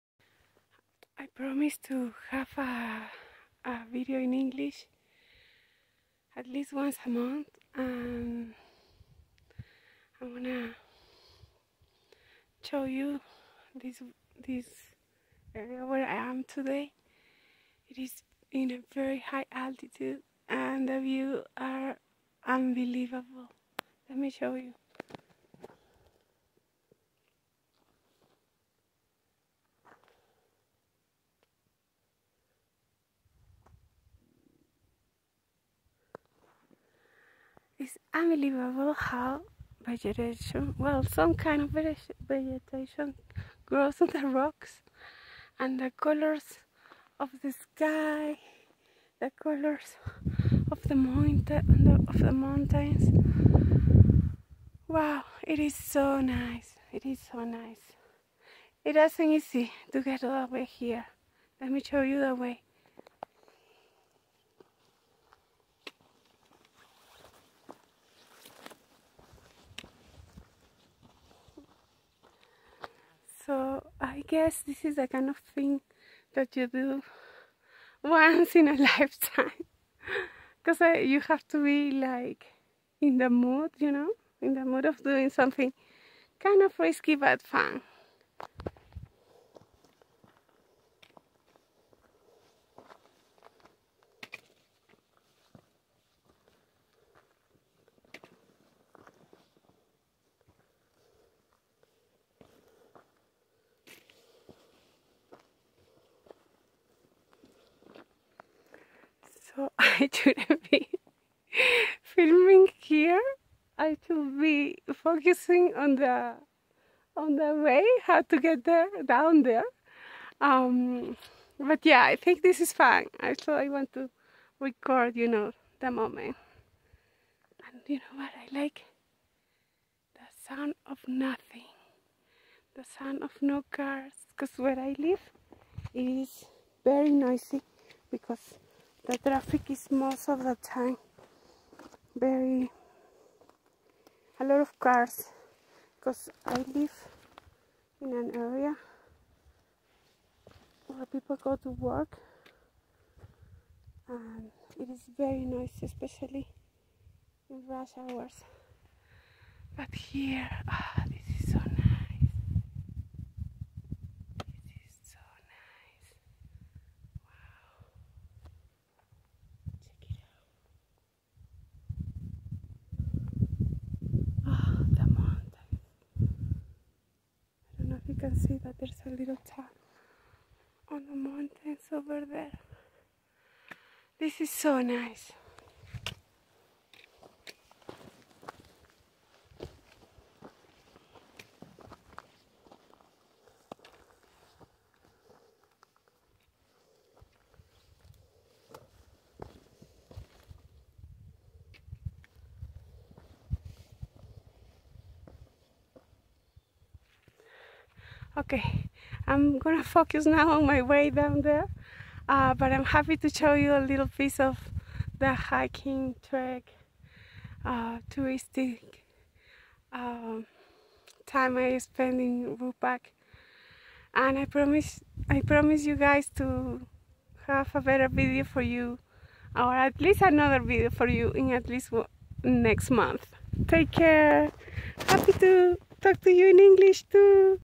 Coming down from Rupac
Coming down from rupac in huaral trying to practice my English skills. Sorry my diction, my grammar and my ability to say something interesting were wrong because the more tired I get the fastest I lose my skills 🤪😁

Here I show you a little bit of the way when I came down from Rupac to Las Pampas.  It is not that easy and you can only go back walking.